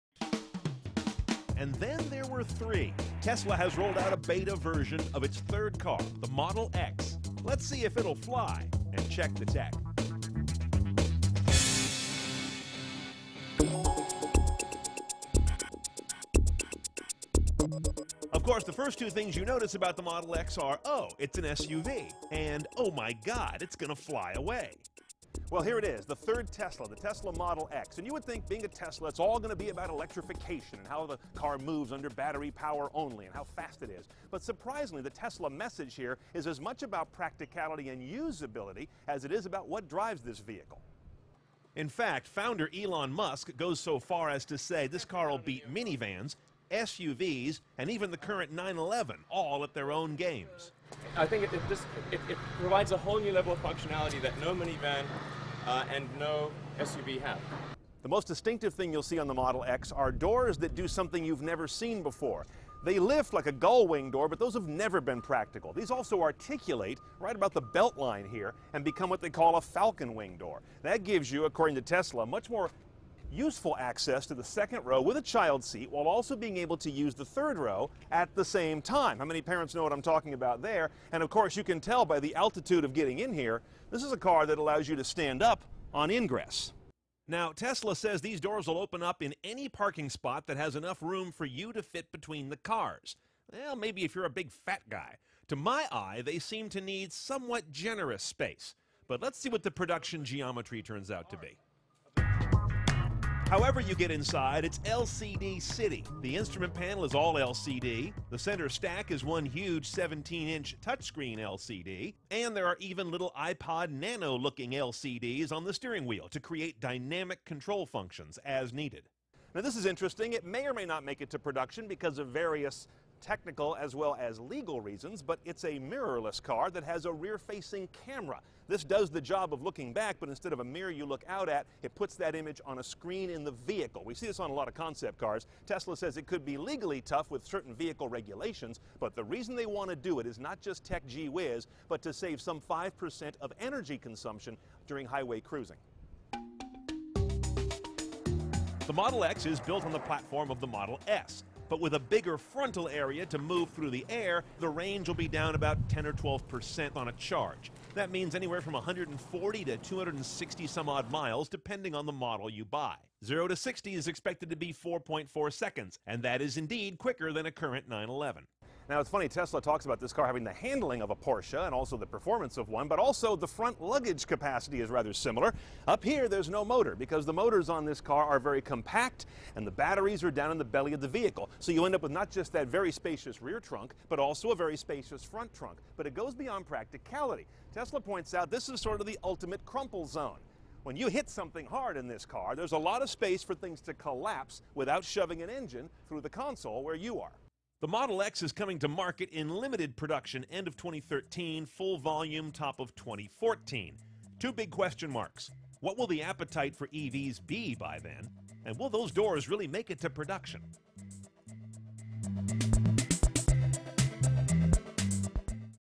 Tesla Motors unveils the Model X
This is the world's first Pure Electric SUV and it's coming 2013... credits Cnet TV